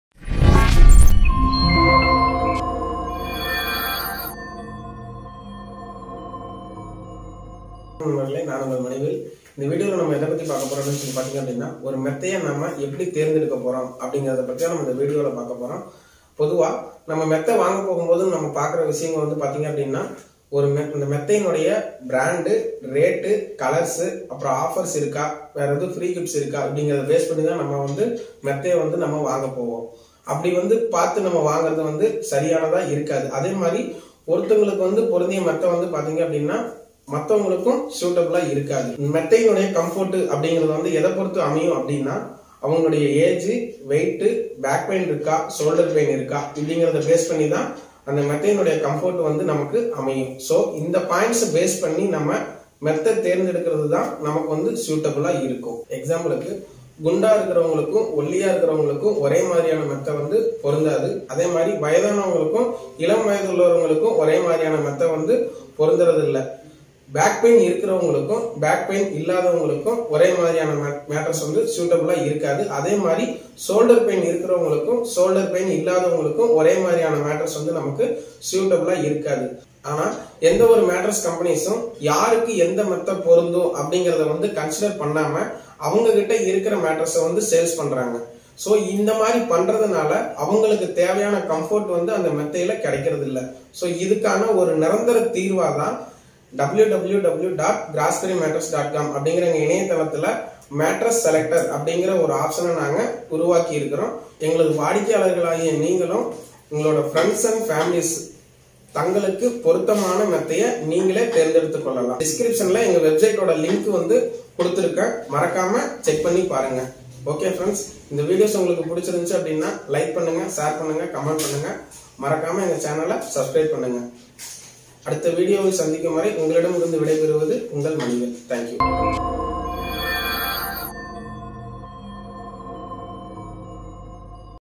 How to Choose The Right Mattress For You in Tamil 2021 by Grassberry Mattress
In This Video You Will Learn , How To Choose The Right Mattress For Your Body.

Grassberry Mattress  Introduced The Mattress Selector, That  Was The Permanent  Solution  For Selecting The Right Mattress For Everyone.